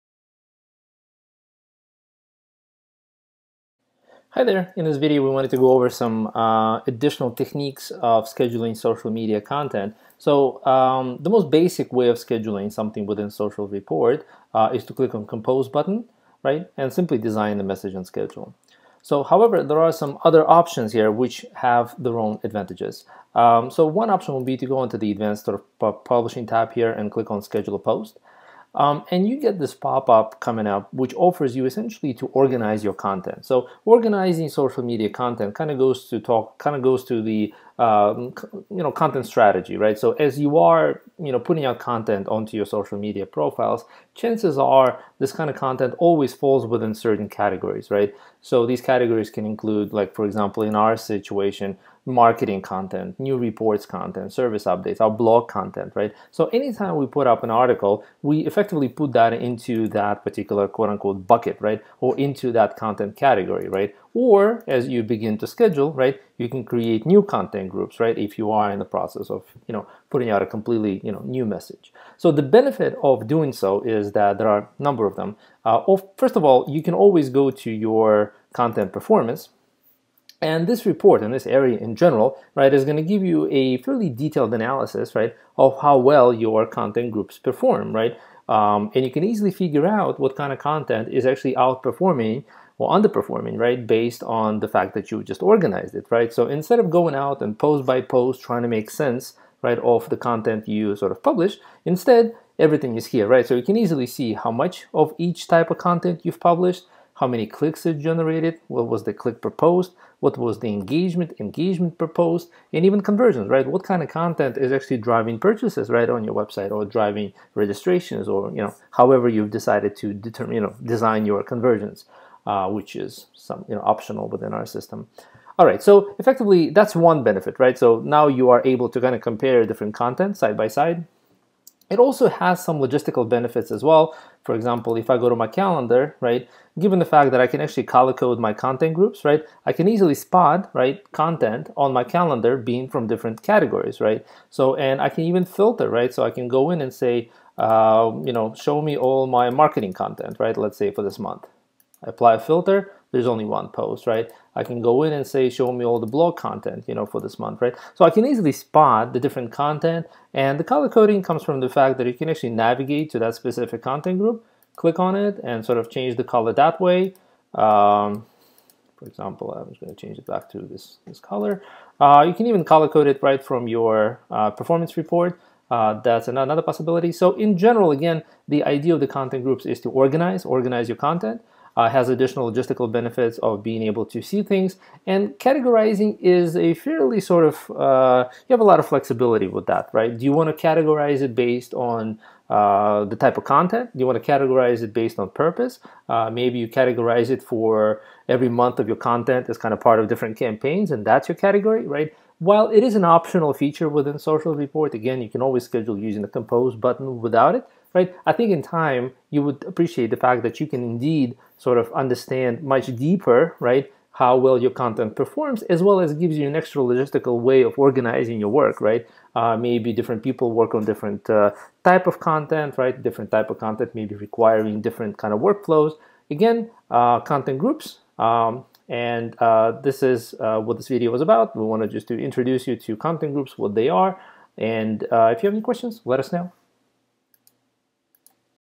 Social Media Posting using Content Groups
This video shows how to better organize your social media scheduling to understand what type of content is resonating best with your audiences.